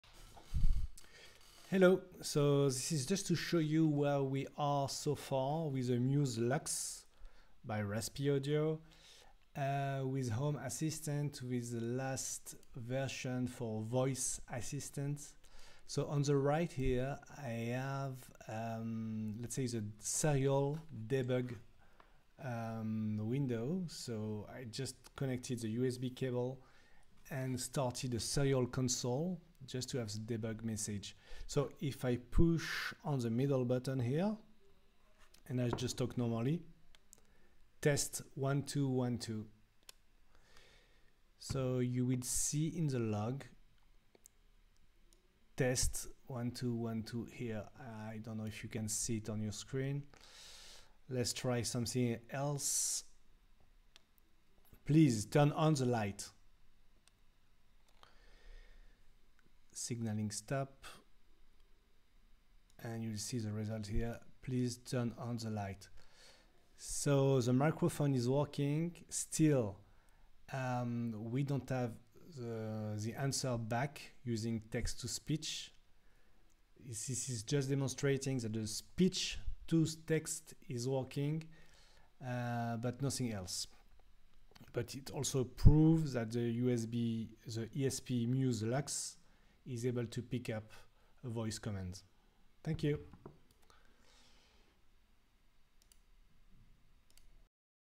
Home Assistant using voice assistant demo using RASPIAUDIO Muse LUXE (ESP32 based speaker)
Demonstrating the Speech to text capabilities of the LUXE using tiny int8 Whisper model in Home Assistant running on Raspberry Pi4.